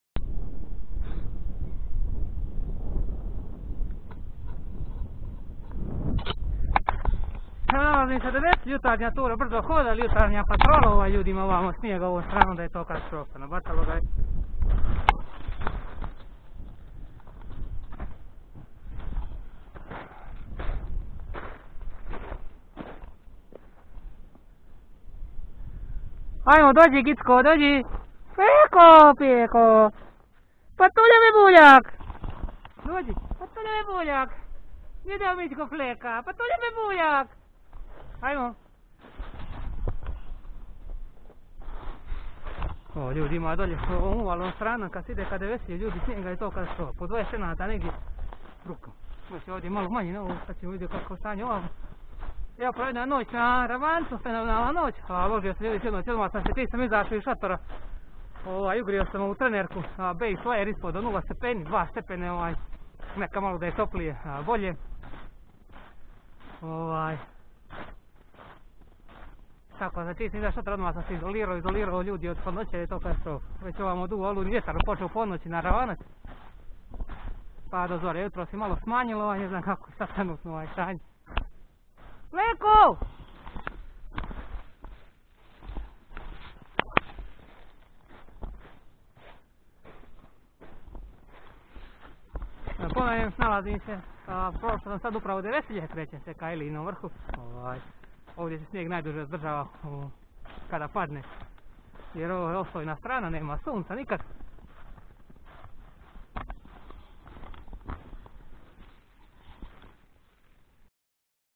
Zimska olujna noć na Ravanac,zalazak-izlazak sunca i 30 km hoda (patrole) 02-03. Februar 2022.
Jutarnja patrola do Devesilja i ilinog vrha sa Flekom ☺